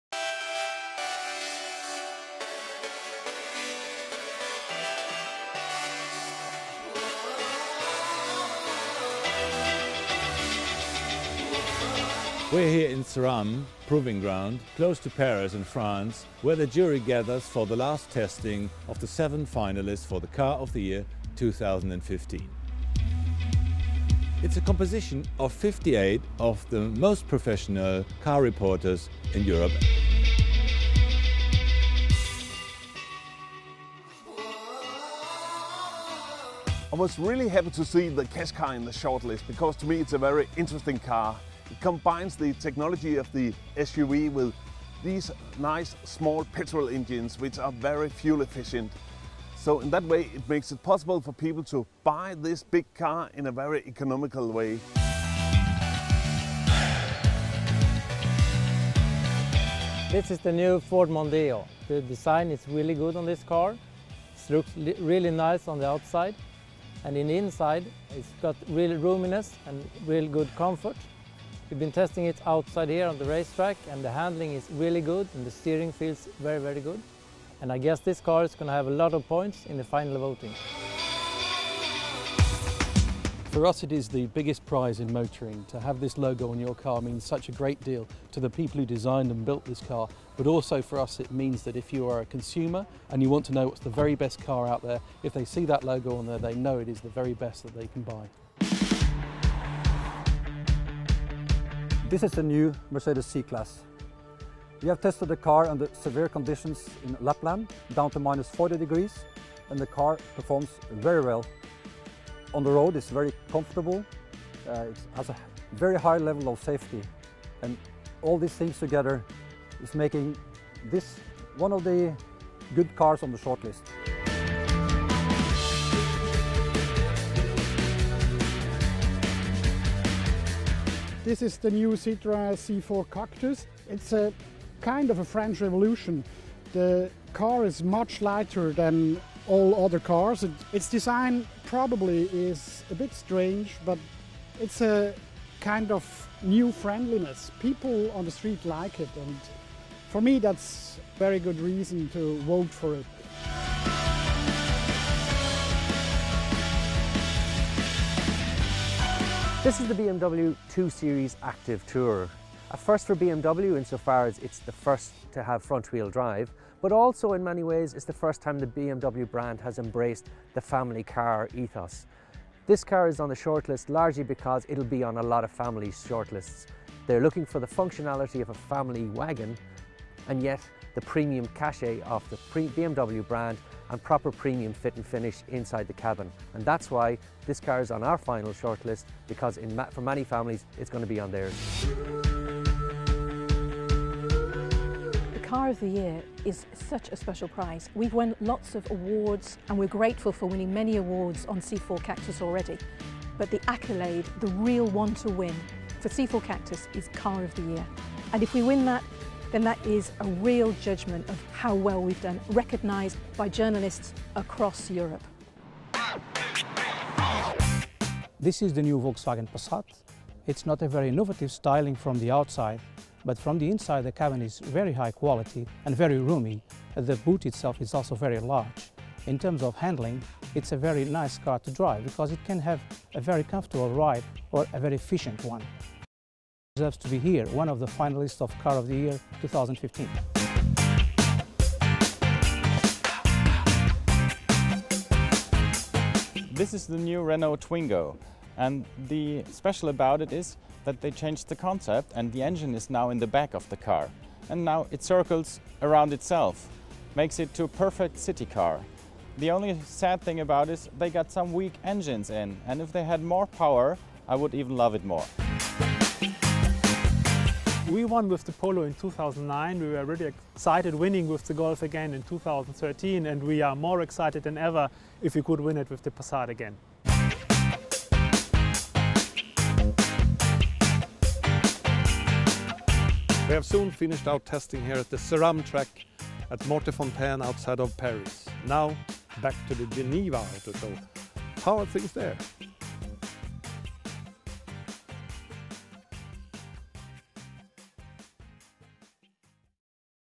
Car of the Year 2015: Mortefontaine
Car of the Year 2015: the nominees, BMW 2-Series Active Tourer, Citroën C4 Cactus, Ford Mondeo, Mercedes C-Class, Nissan Qashqai, Renault Twingo, Volkswagen Passat, at Mortefontaine.